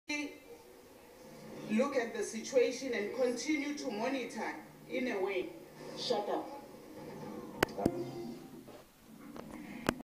War of words: Susan Shabangu
Minister of Mineral Resources in South Africa tells fellow parliamentarian to "shut up"